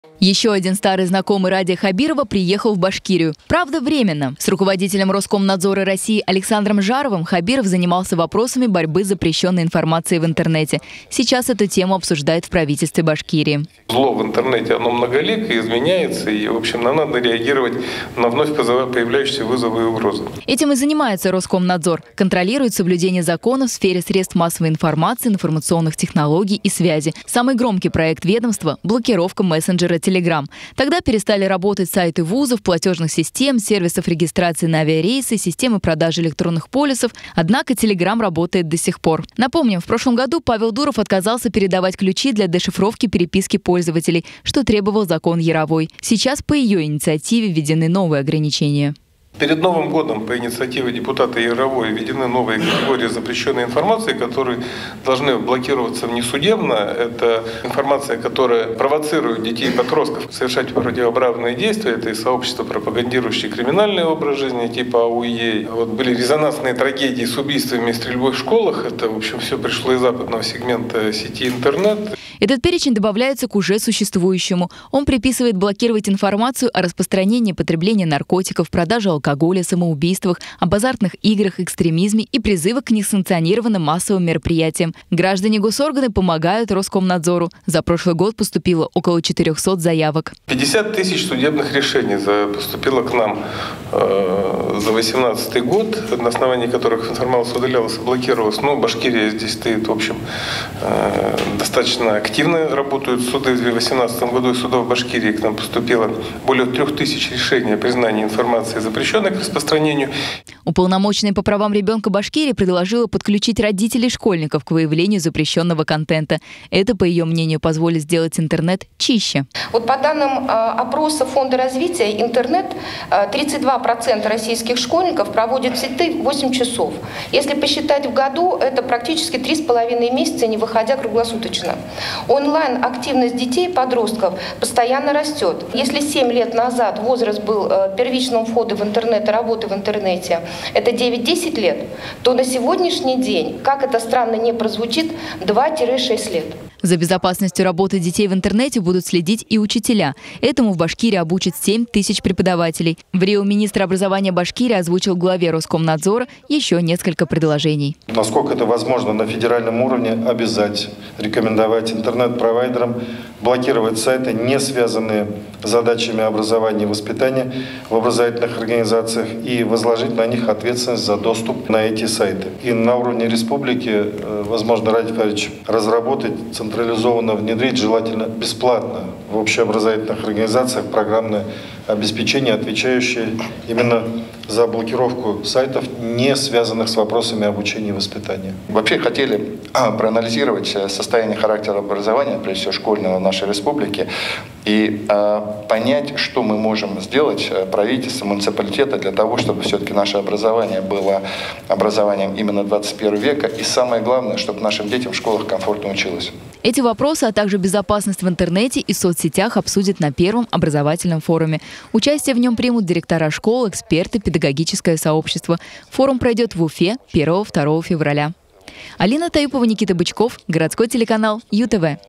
UTV. Главный борец с мессенджером Telegram Александр Жаров в Уфе встретился с Радием Хабировым
Еще один старый знакомый Радия Хабирова приехал в Башкирию. Правда, временно. С руководителем Роскомнадзора России Александром Жаровым Хабиров занимался вопросами борьбы с запрещенной информацией в интернете. Сейчас эту тему обсуждают в Правительстве Башкирии.